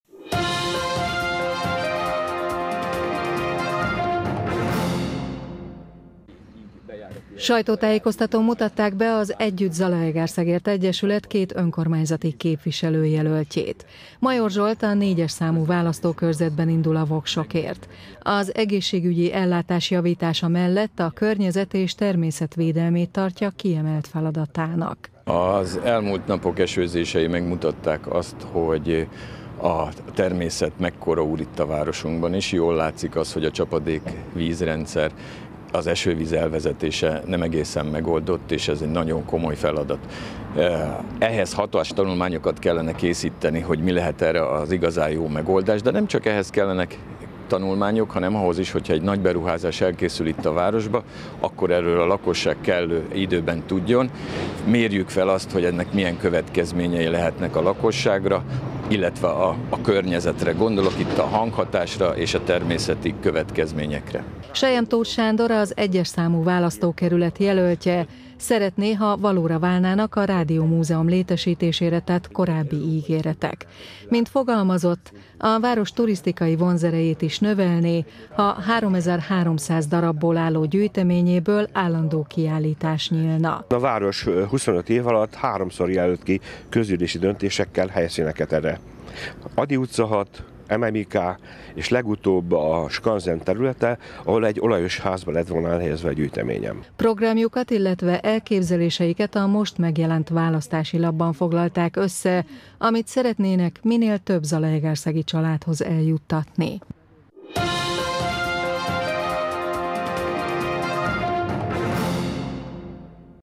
Választás 2024 - Bemutatkoztak az EZE jelöltjei – ZTV Híradó 2024-06-06
Sajtótájékoztatón mutatták be az Együtt Zalaegerszegért Egyesület két önkormányzati képviselőjelöltjét. Major Zsolt a 4. számú választókörzetben indul a voksokért. Az egészségi ellátás javítása mellett a környezet- és természet védelmét tartja kiemelt feladatának.